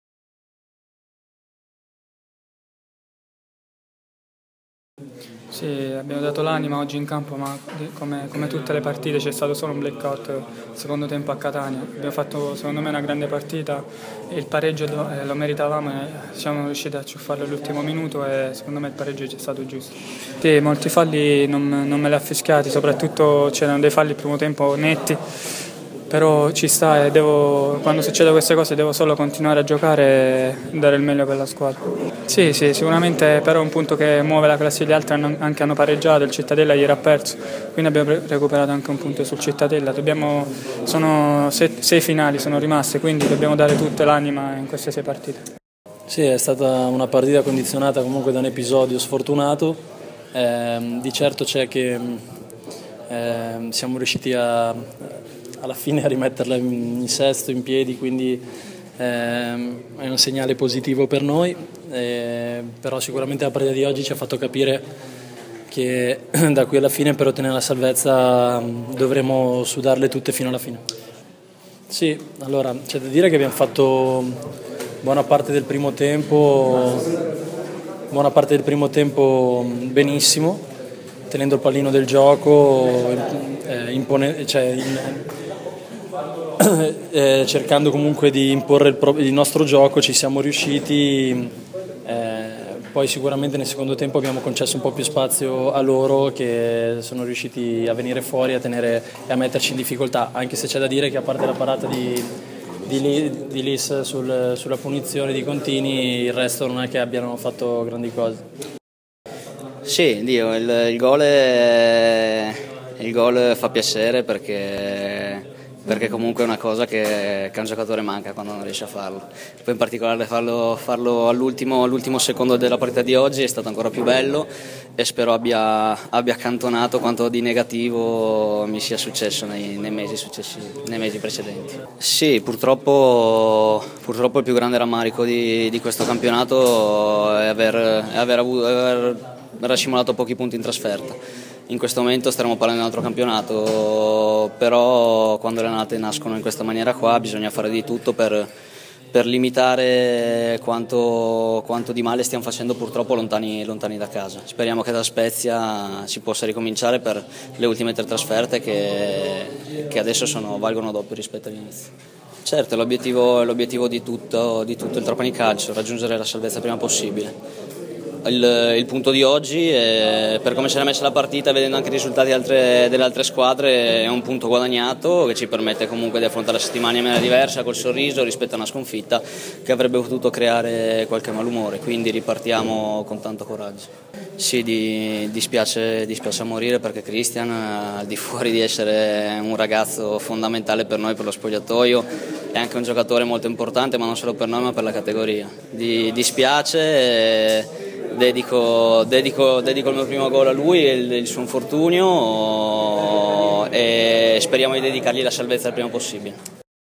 Trapani-Bari 1-1. Falco, Perticone e Scozzarella dopo la gara 18.04.15 ©TrapaniCalcio.it
Trapani-Bari 1-1: le dichiarazioni post partita dei calciatori granata Filippo Falco, Romano Perticone e Matteo Scozzarella. 36a Giornata serie B 2014/15: Sabato 18.04.15. A cura dell'Ufficio Stampa del Trapani. ©TrapaniCalcio.it